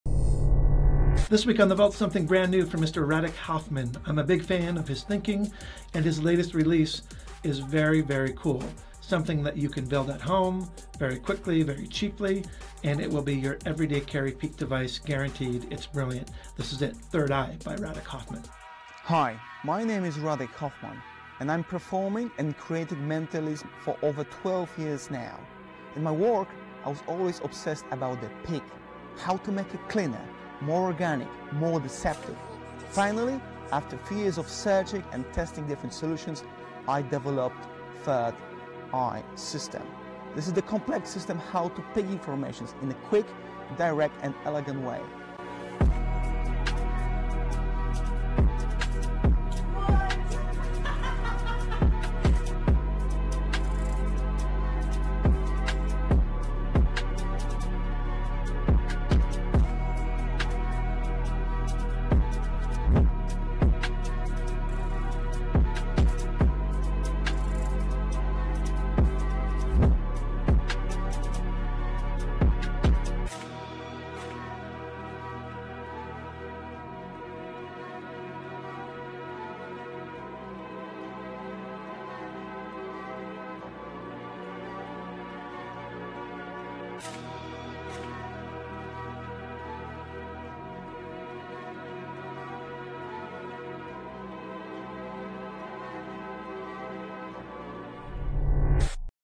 The Vault - Third Eye by Radek Hoffman
"No electronics or gizmos here. Just a simple, direct and easy technique that once learned will serve you for a lifetime, enabling you to read minds whenever you want! This is the type of mentalism I love the most!"
- Marc Paul

This is not just one trick, it is a complex system of how to peek at information written down by your spectators. It is very elegant, clean, and extremely effective. After learning this system you will probably throw away all your peeking devices and stick with the Third Eye system. You will see how to do perfect peeks, how to make them invisible and impossible. You will also learn a few full routines so you can apply this system to your close-up or stage act.

Peeking wallets, boxes and other strange props should go away! If you really can read minds, all you need is a piece of paper and a pen.

This is a deep dive into how to peek at information in the most efficient way but also how to reveal it in an interesting way. How to make it deceptive and engaging for your audience. Be ready for Third Eye and read minds like never before!

KEY FEATURES:

    Very organic and elegant at the same time!
    Easy to do!
    Reset in 5 seconds!
    All needed gimmicks, You can do it easily by yourself!

Have a look at what one of the best mentalists says about this project:

"Casual, convincing, practical. Third Eye combines tried and tested techniques, applying them in effective ways to create something that is simple and deceptive. It's perfect as part of an EDC, and even for professional use."
- Nique Tan

"Radek's attention to detail and thoughtful structuring has completely revolutionised this old principle into a modern gem."
- Mark Lemon